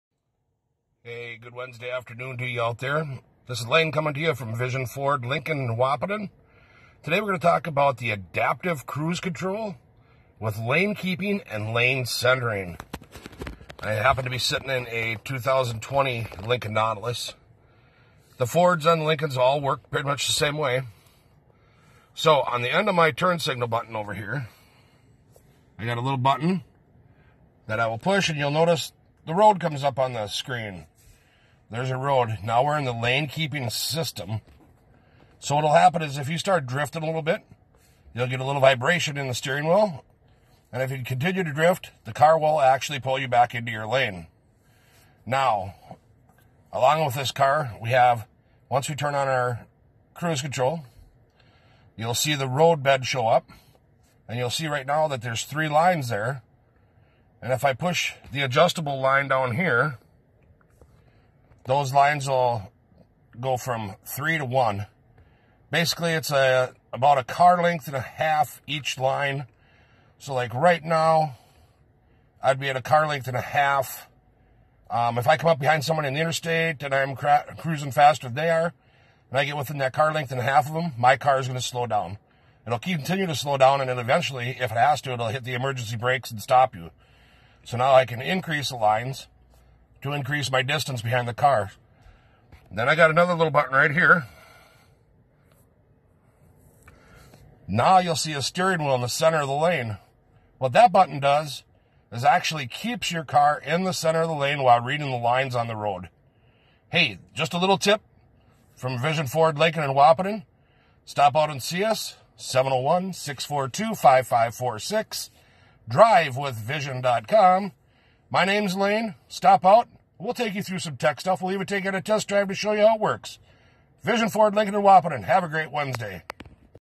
Lincoln Nautilus Adaptive Cruise Control Tutorial
Learn how to operate your adaptive cruise control in your Lincoln Nautilus and other Lincoln vehicles that have this feature.